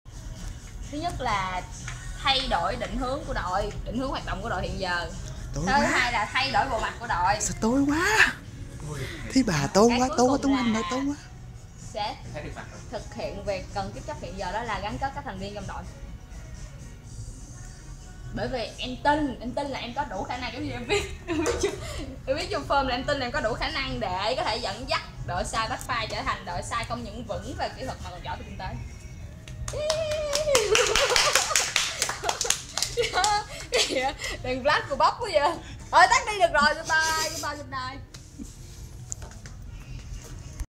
Phát biểu nhận chức của President 2014
Phát biểu nhận chức của bạn Phạm Lê Diệu Hiền.
President nhiệm kỳ 2014 của Enactus HCMUT.
Địa điểm: ABC Bakery, 3 Tháng 2 Quận 10